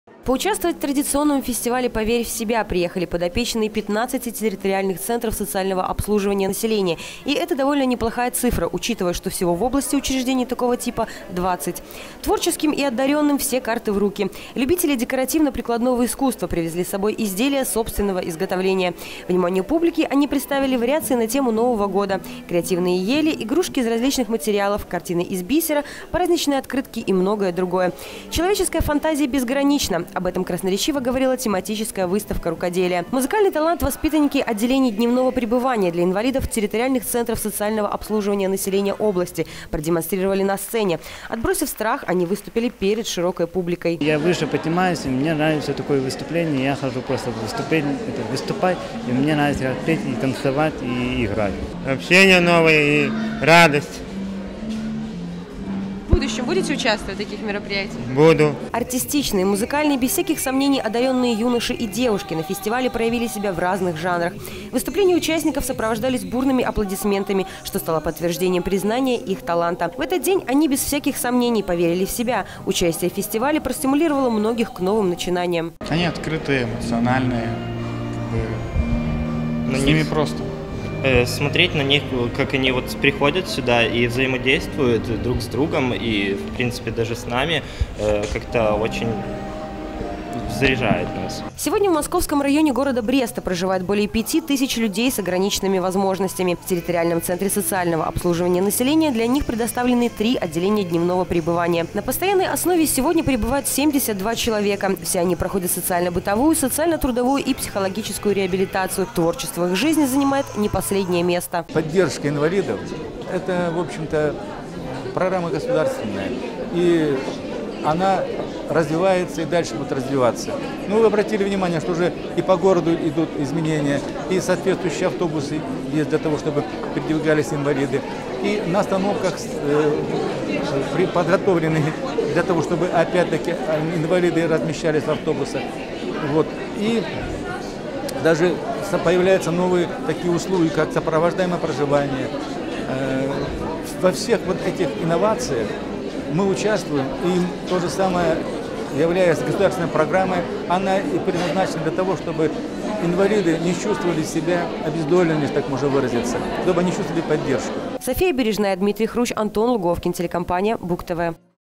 2015-12-03 г. Брест. Международный день инвалидов: фестиваль «Поверь в себя». Телекомпания Буг-ТВ.
В Республике Беларусь социальная защита людей с различной степенью инвалидности – одно из приоритетных направлений государственной политики. Наверняка поэтому наша страна поддержала инициативу проведенияМеждународного  Дня инвалидов, который мировая общественность отмечает  3 декабря. Сегодня в Беларуси проживает более пятисот тысяч людей с ограниченными возможностями.
В этот день не будет лишним напомнить о том, что среди нас живут люди, заслуживающие не только сочувствия, но прежде всего поддержки, обеспечения их прав на достойную жизнь, труд и отдых. В канун Международного дня инвалидов в Бресте по инициативе Территориального центра социального обслуживания населения Московского района прошёл четвертый областной ежегодный фестиваль инвалидов «Поверь в себя!».Его программа расширилась, увеличилось и количество желающих показать себя и свои способности. Чем в этом году удивляли участники фестиваля «Поверь в себя»,   чем порадовали организаторы, расскажет наш корреспондент.